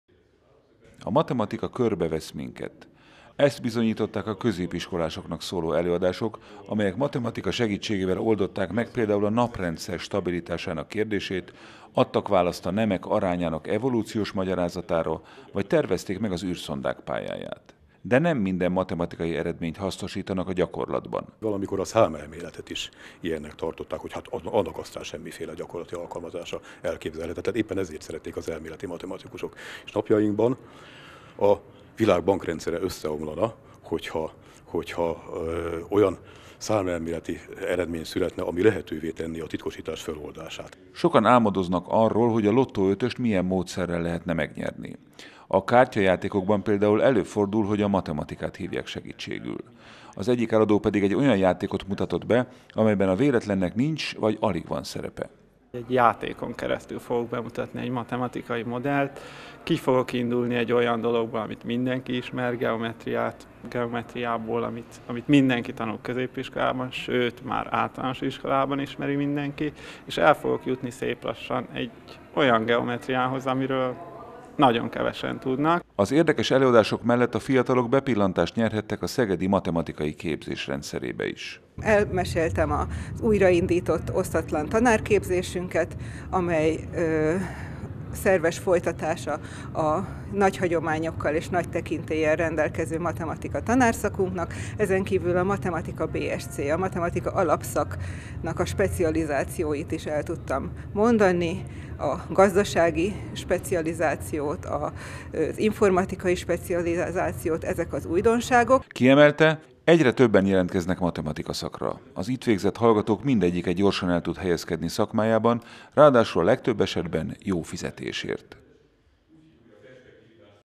Szegedi Hírek A matematika érdekes és izgalmas 2016.04.26.
15 hazai középiskolából érkeztek diákok arra a matematikai előadássorozatra, amelyet 12. alkalommal rendeztek meg a Szegedi Tudományegyetem Bolyai Intézetében. Az Egyetemi Tavasz elnevezésű esemény célja, hogy megmutassák a matematika érdekes és izgalmas, valamint felhívják a figyelmet a szegedi magas szintű oktatásra. A matematika körbevesz minket. Ezt bizonyították a középiskolásoknak szóló előadások, amelyek matematika segítségével oldották meg például a Naprendszer stabilitásának kérdését, adtak választ a nemek arányának evolúciós magyarázatára vagy tervezték meg az űrszondák pályáját. De nem minden matematikai eredményt hasznosítanak a gyakorlatban. Az érdekes előadások mellett a fiatalok bepillantást nyerhettek a szegedi matematika képzés rendszerébe is. Egyre többen jelentkeznek matematika szakra. Az itt végzett hallgatók mindegyike gyorsan el tud helyezkedni a szakmájában, ráadásul- a legtöbb esetben- jó fizetésért.